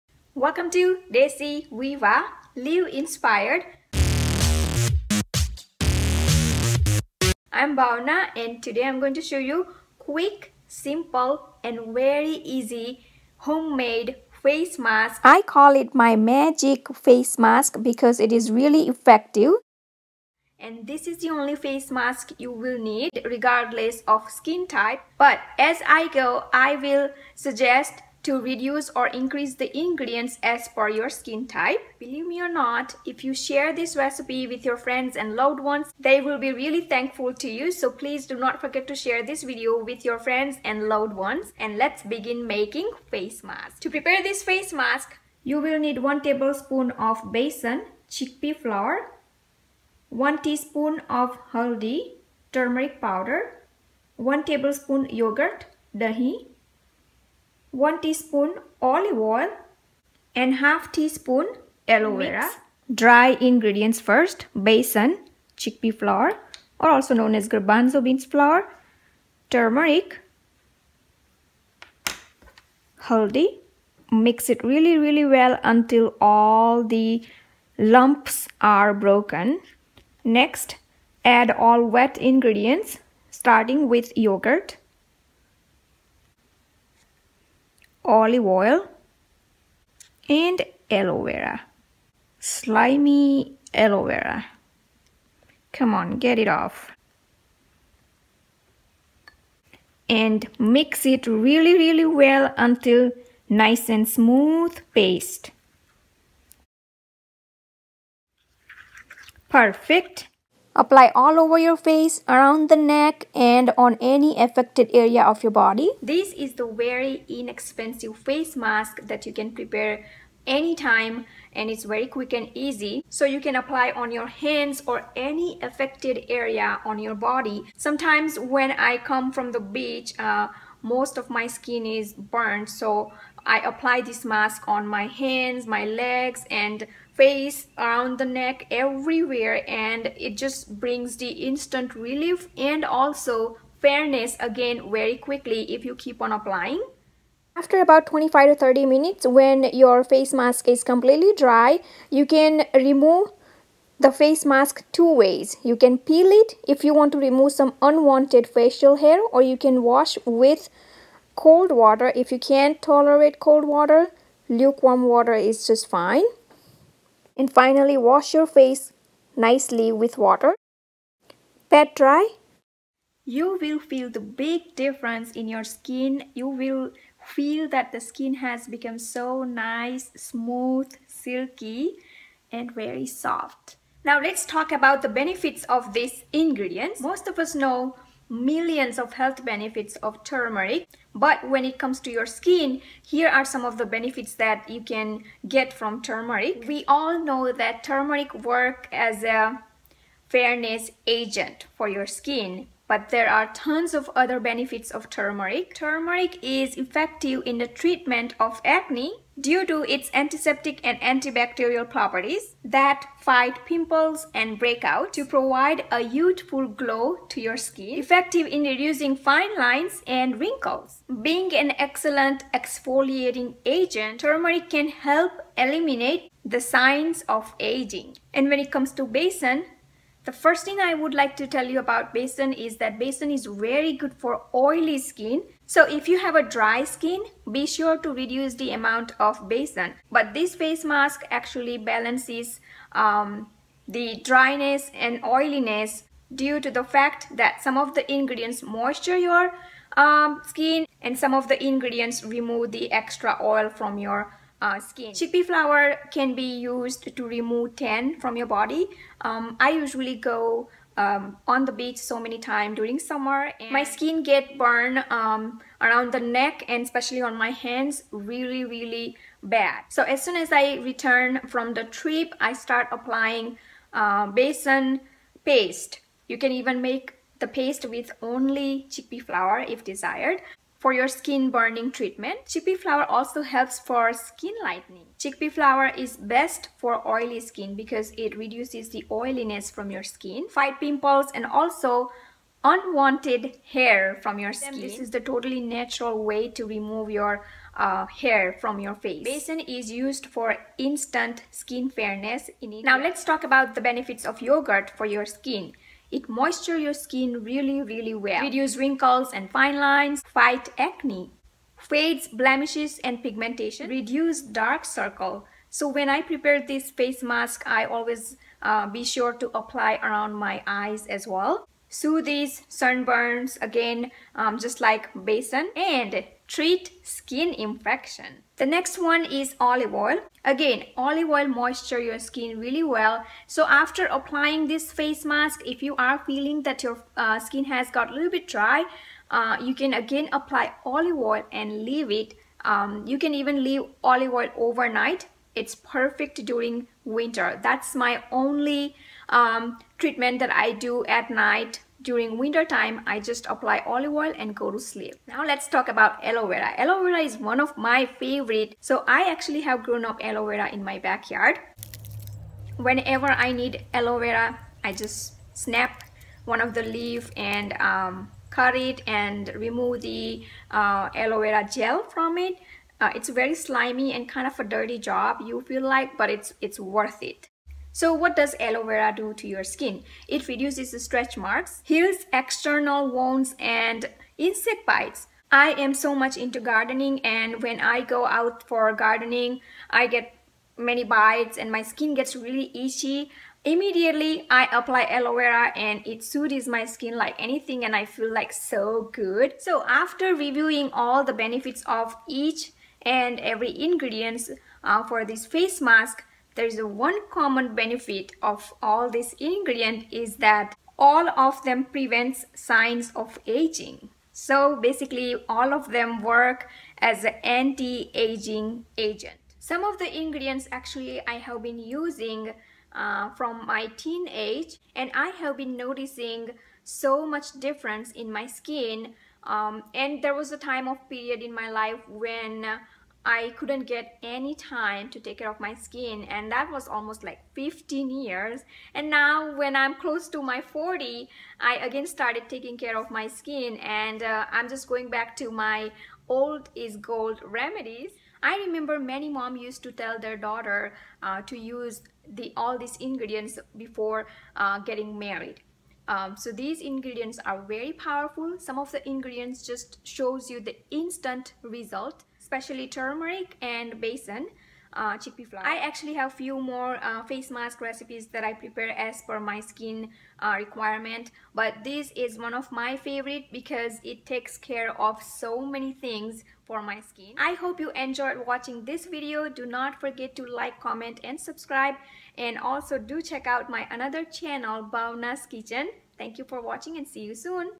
Magic DIY Homemade Face Mask for all skin type | Skin Glowing, Reduce Wrinkles
The Only Face Mask (DIY Homemade) you will ever need for any skin type!
Benefits:
Glow skin in just one use
reduce wrinkles
remove sun tan, acne, blackheads, unwanted facial hair
Perfect to make your skin soft, silky and flawless!